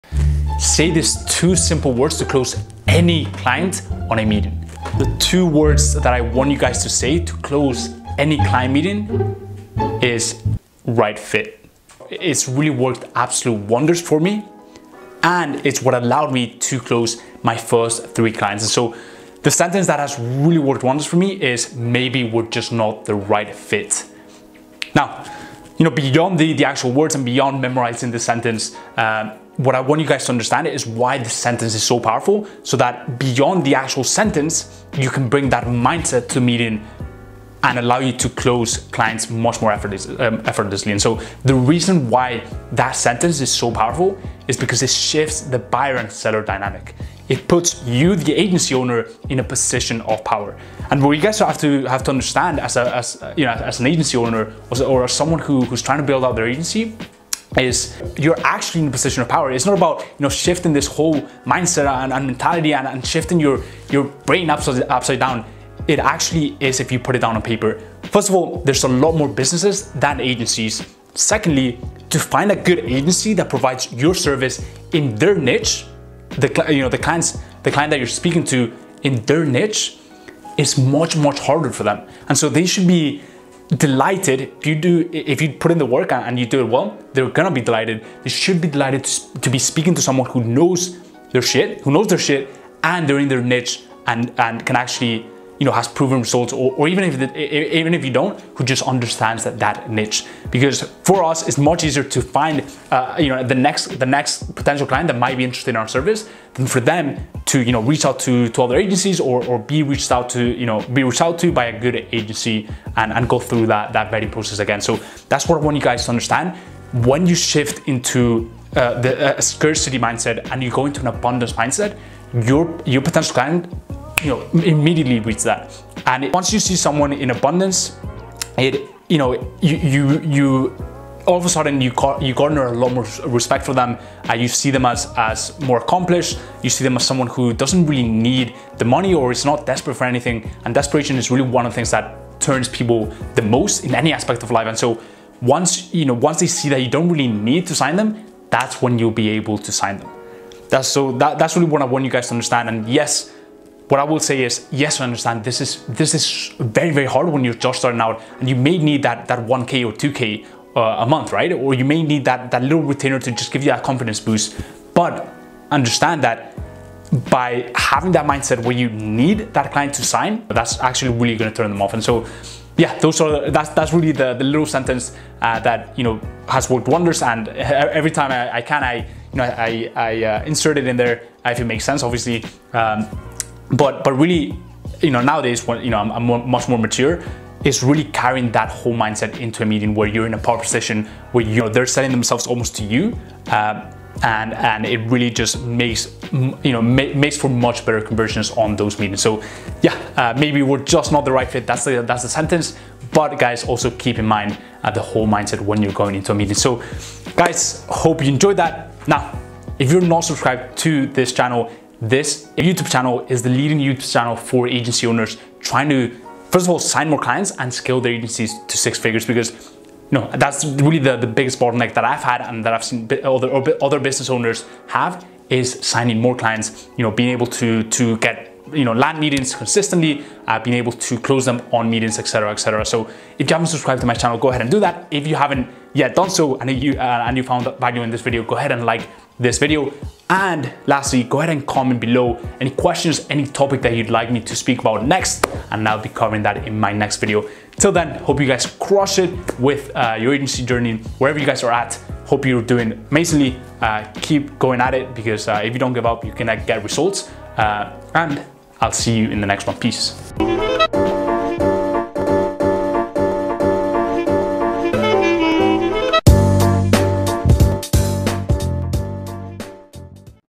How to Close ANY Client on a Meeting (JUST SAY THIS!) - Agency & SMMA Outreach 2020
Client meetings can be tough, especially when you are JUST starting out. Landing meetings & signing new clients is BY FAR the biggest bottleneck I see agency owners have when trying to grow their agency.

So when you get a meeting, pressure is onto close. However, what if I told you that that is EXACTLY what is preventing you from signing them? When I started saying the two words I share with you in this video, my conversion rates went through the ROOF. You're not going to want to miss it!

Second YouTube Channel: Jaime Higuera SMMA